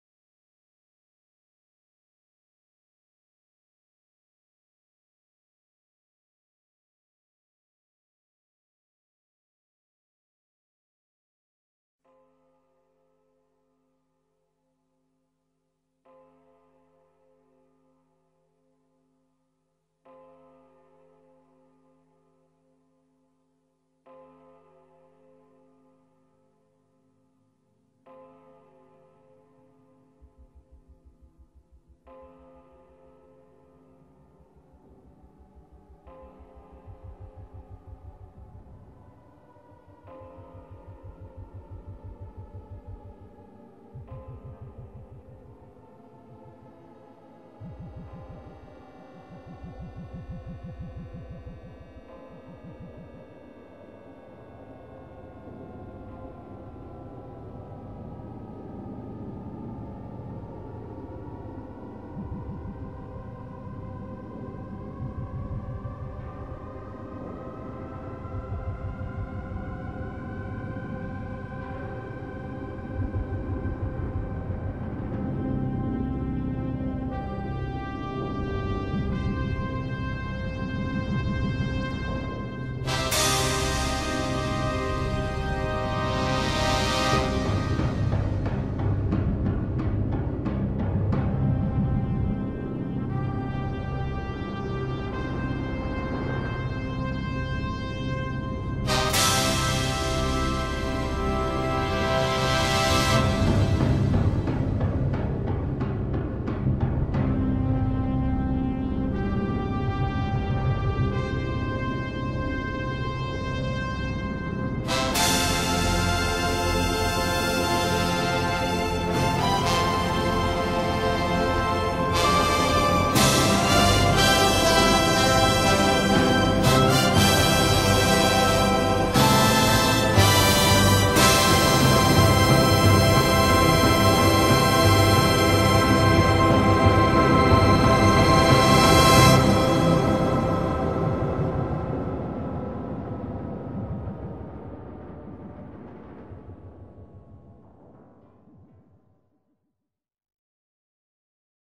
Preserved Northern General MCW Metrobus
Former Northern General MCW Metrobus, 3501 UTN501Y, having been restored in poppy red.